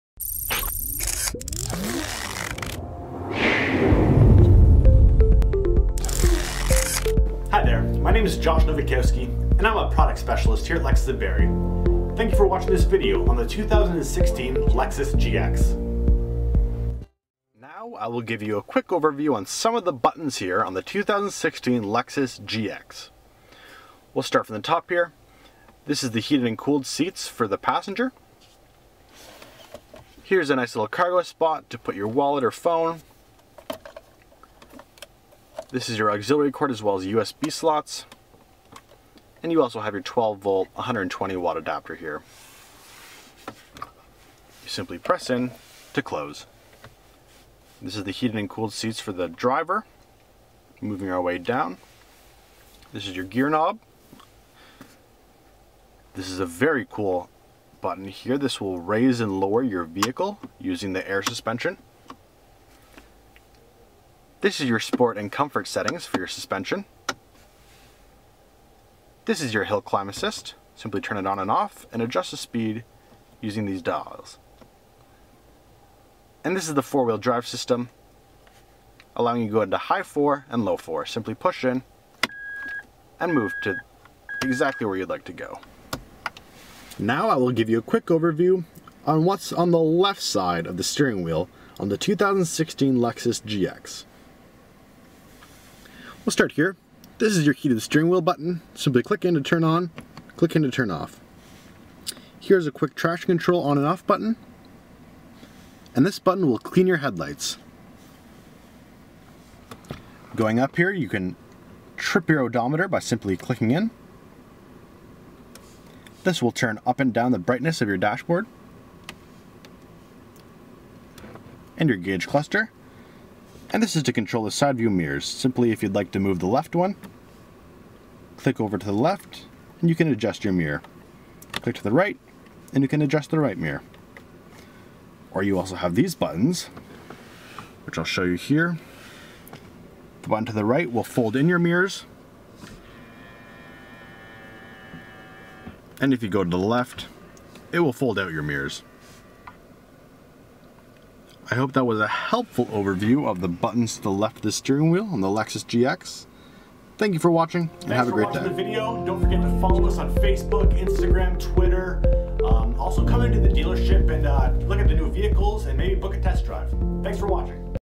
Lexus GX Interior Buttons
Hey everyone Josh Novakowski here. Today I am going to give you a quick look and explanation of some of the interior buttons on the 2016 Lexus GX